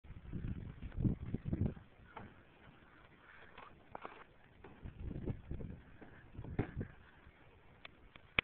Backflip Off Porch
I do a backflip off my porch.